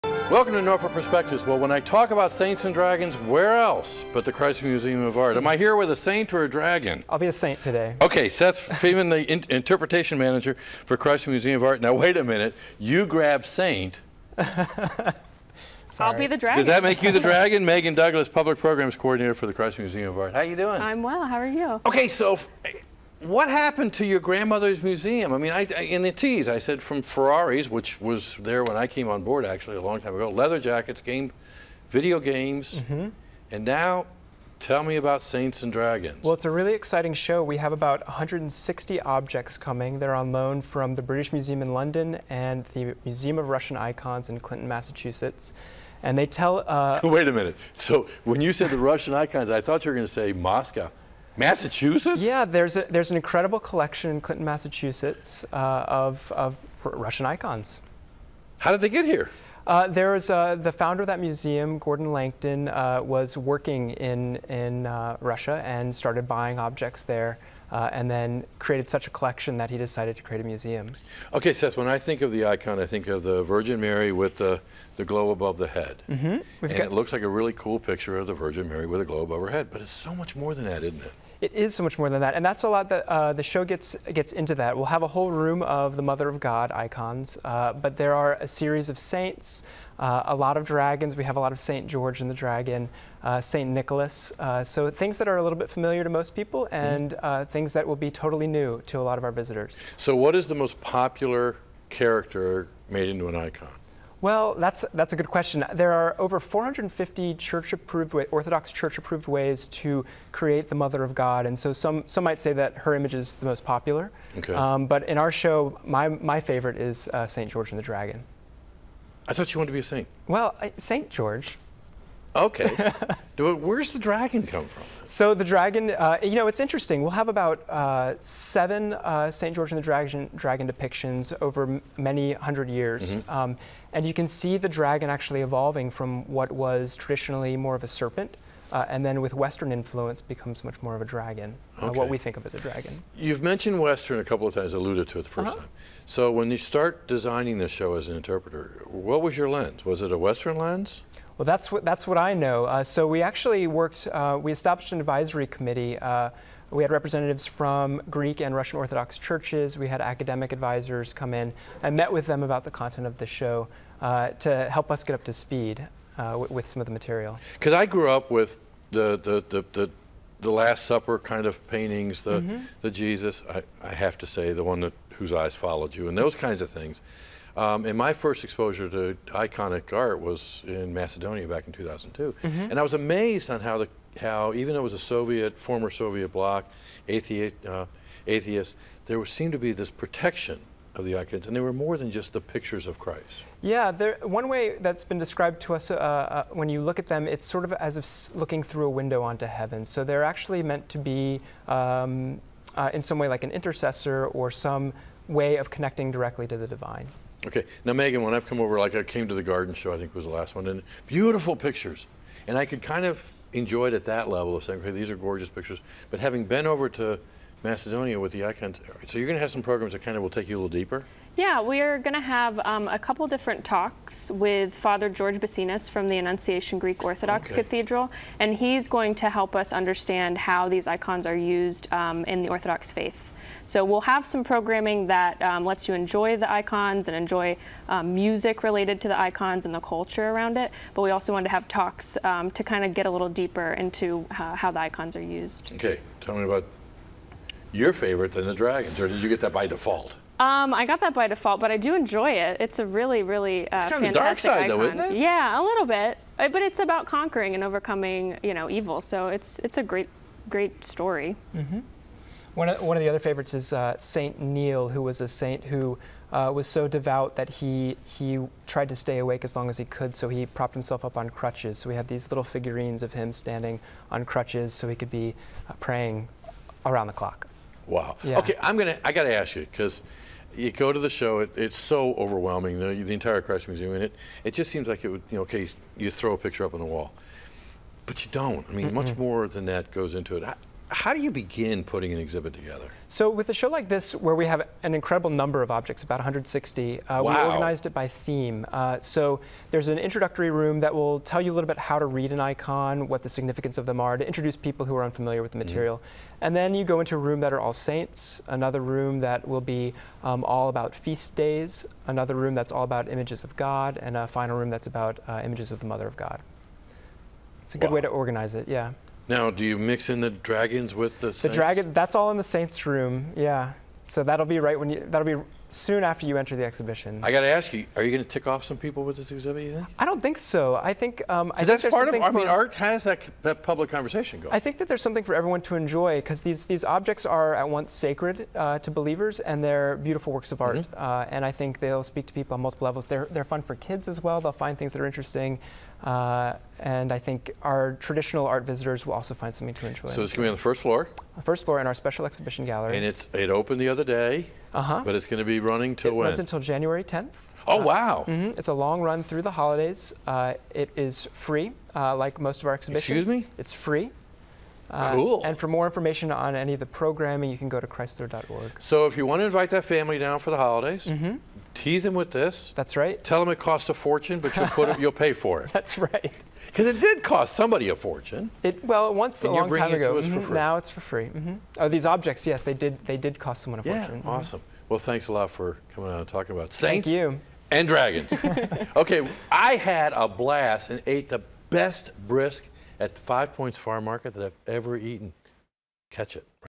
Saints & Dragons @ Chrysler Museum
Saints and Dragons
Icons from Byzantium to Russia
Chrysler Museum of Art
 160 extraordinary works of religious, historical, and artistic importance—rare icons and extraordinary artifacts that rarely leave the collections of The British Museum, London, and the Museum of Russian Icons, Clinton, Mass.